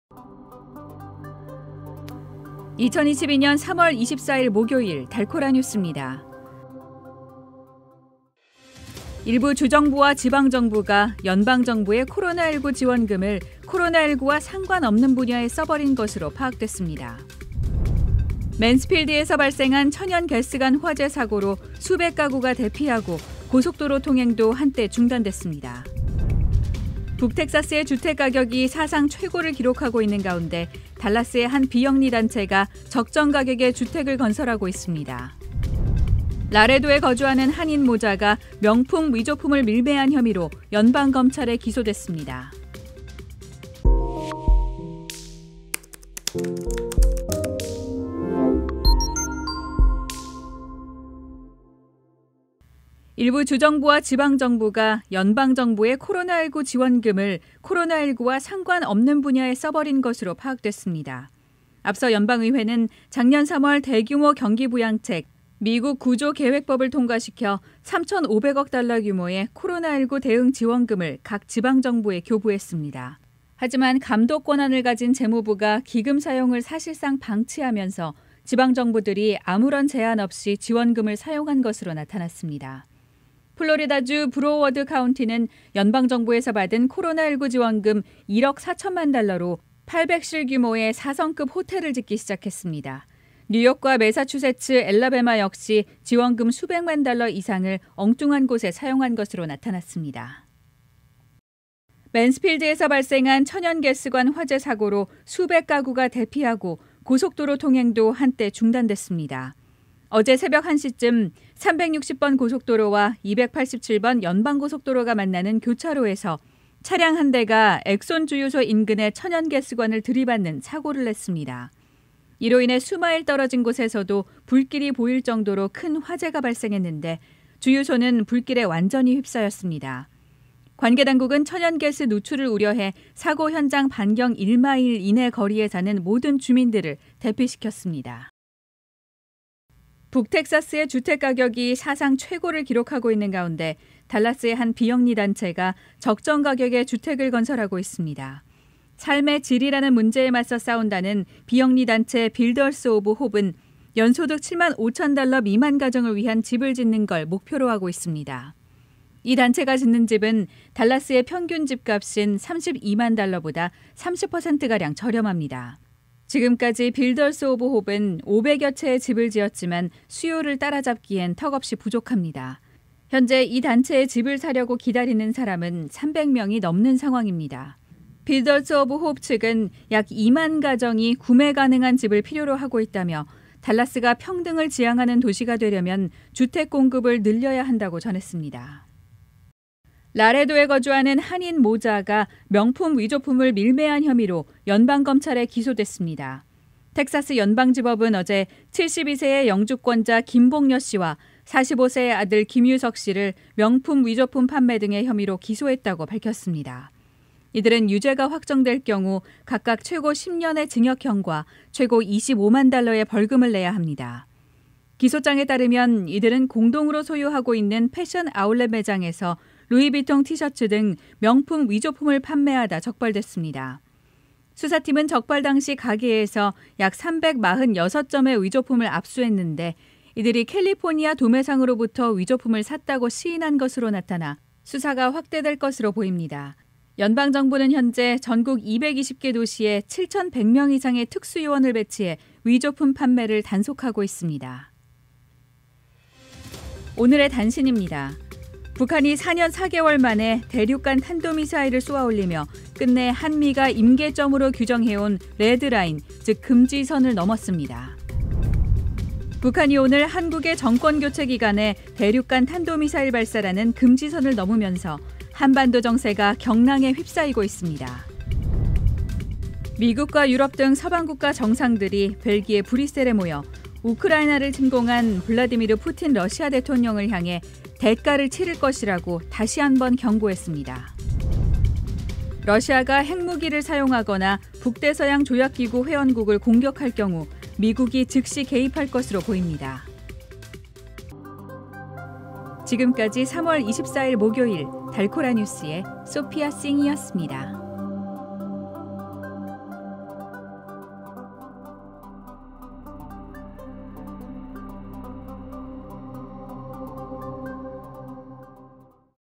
코로나 19 지원금, 엉뚱한 곳으로 술술 샜다 / 텍사스 맨스필드 천연 개스관 대형 화재 발생 / 명풍 짝퉁 팔다가 덜미, 라레도 한인 모자 기소 [3월 24일 달코라영상뉴스]
[3월 24일 달코라영상뉴스]
- 코로나 19 지원금, 엉뚱한 곳으로 술술 샜다
- 텍사스 맨스필드 천연 개스관 대형 화재 발생
- 달라스, 적정 가격의 주택 건설 필요하다
- 명풍 짝퉁 팔다가 덜미, 라레도 한인 모자 기소

미국 중남부 유일의 한인 라디오 방송 DKNET 달라스 코리안 라디오!
달라스 지역에선 공중파 라디오 AM 730으로~
언제 어디서나 DKNET 모바일 앱으로~